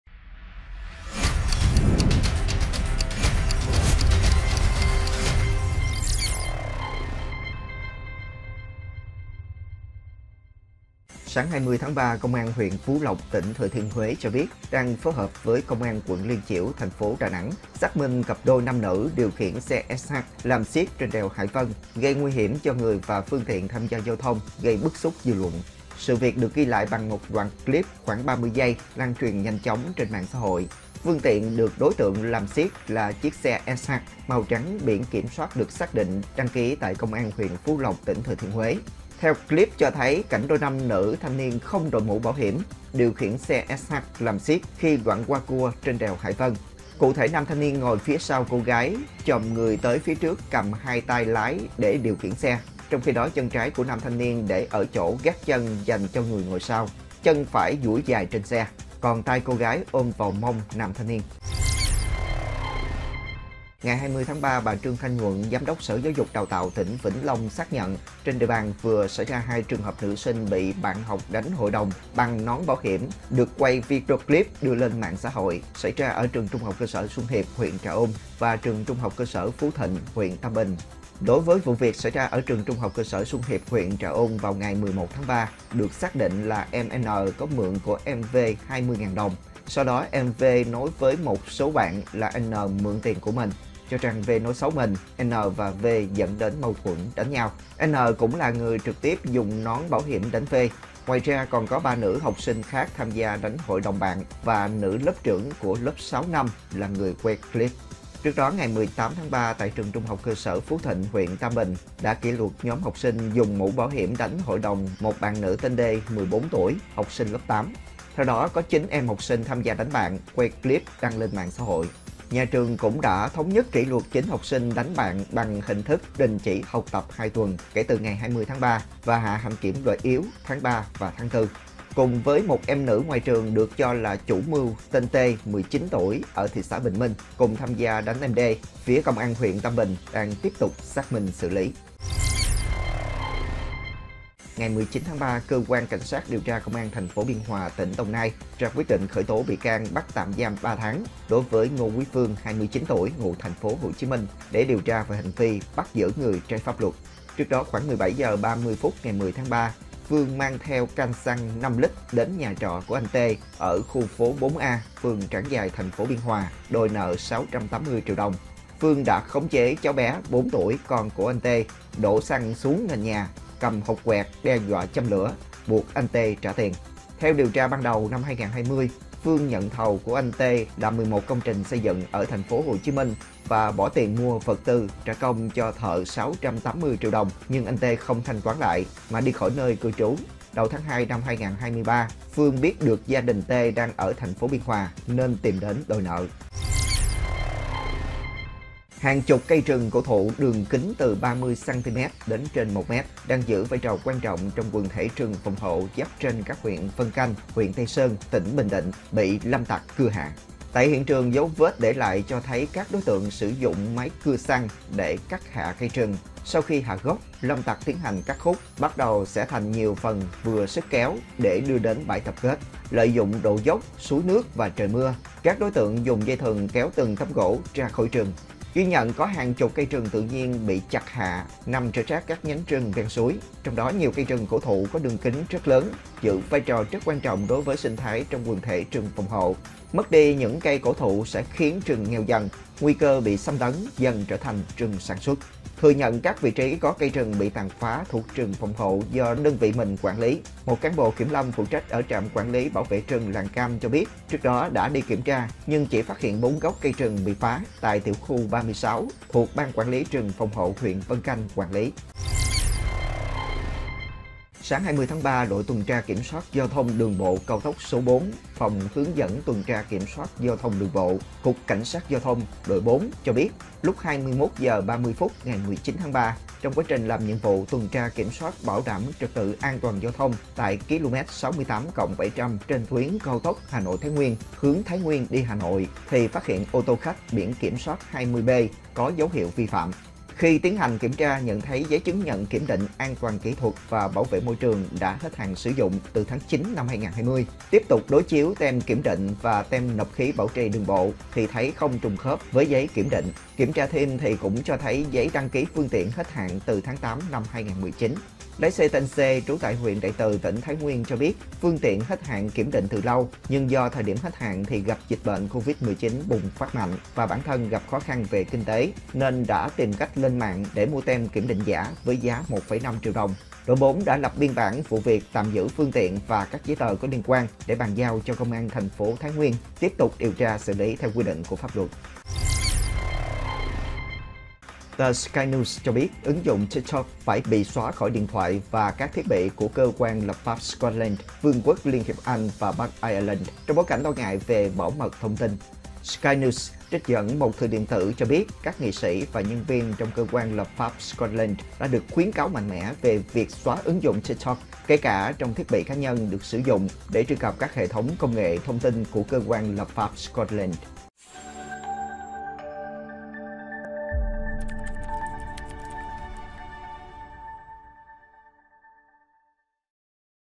Đôi nam nữ làm “xiếc” bằng xe SH trên đèo Hải Vân I Tin nóng 12H
Tin nóng buổi trưa của SGGPO ngày 20-3 có các thông tin: Điều tra, xử lý đôi nam nữ làm “xiếc” bằng xe SH trên đèo Hải Vân; Liên tiếp xảy ra nữ sinh bị đánh hội đồng ở Vĩnh Long; Nhiều cây cổ thụ bị "xẻ thịt" giữa rừng phòng hộ ở Bình Định…